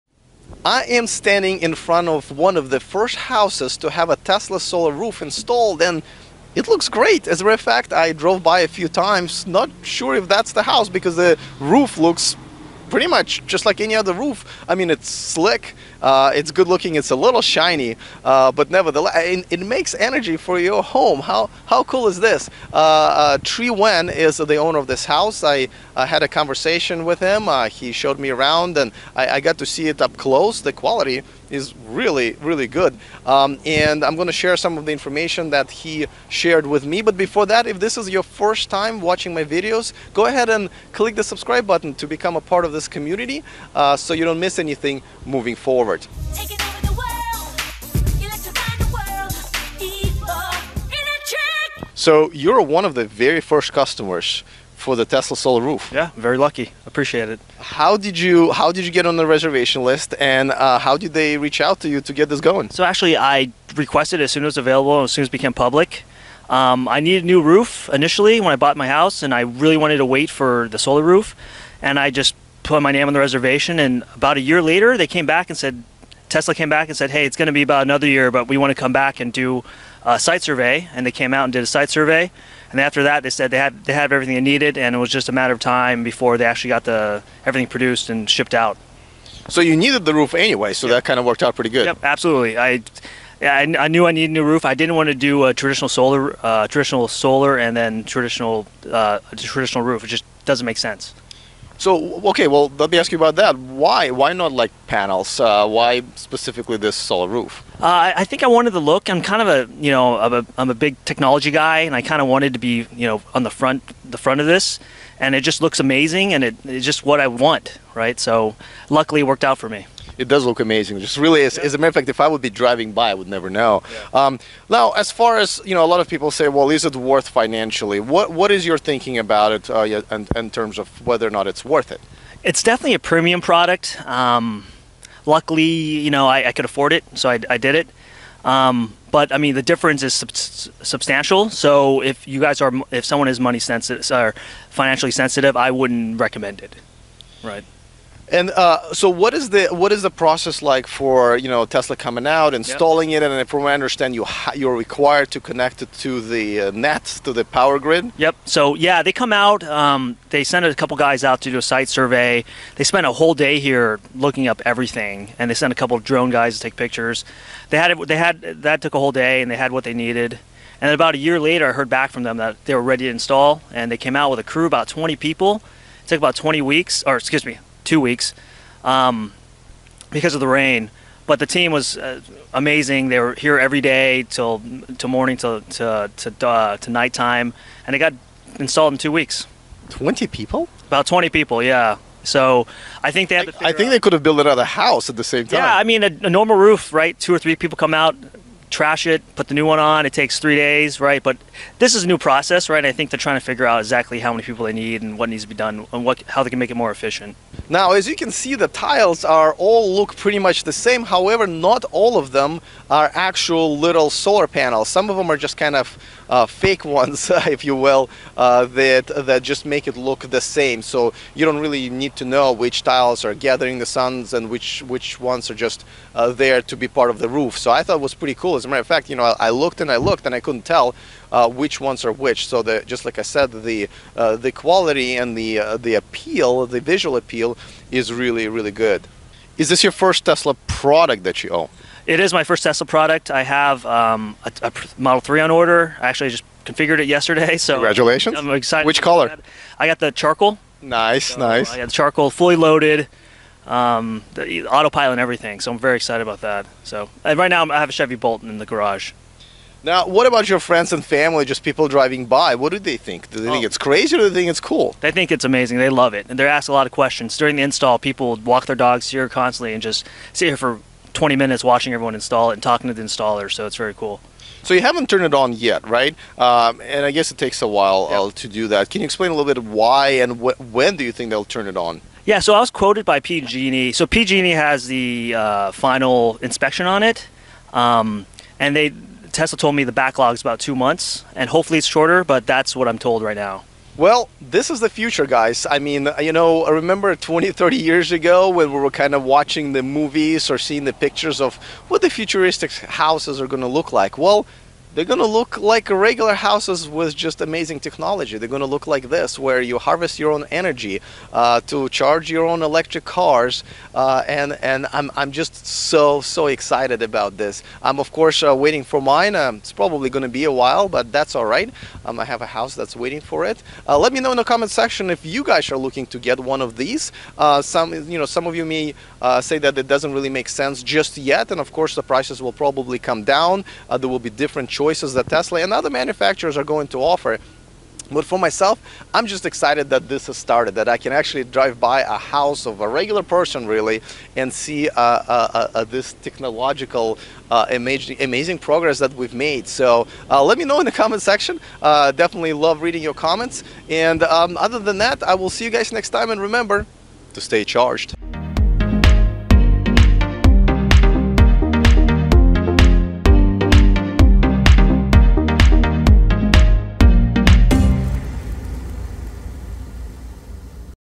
Tesla Solar Roof: A Rare First Look
A rare look at one of the first installations of Tesla Solar Roof in California including the interview with the owners, great footage of the solar roof’s tiles / shingles and my overall impression and review of this amazing product.

Amazon products EV owners love: